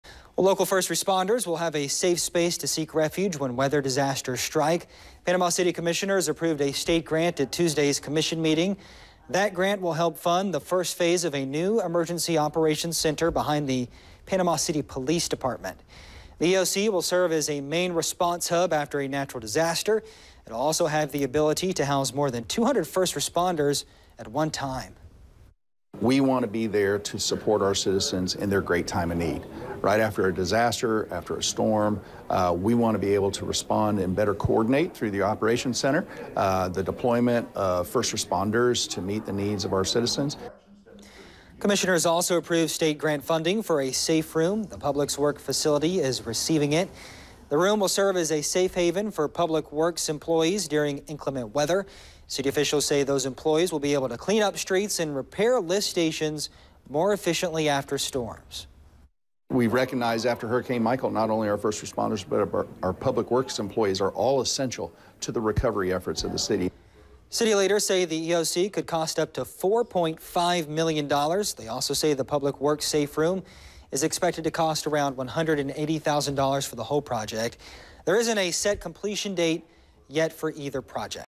EOC and Public Works Safe Building
Local first responders will have a safe space to seek refuge when weather disaster strikes.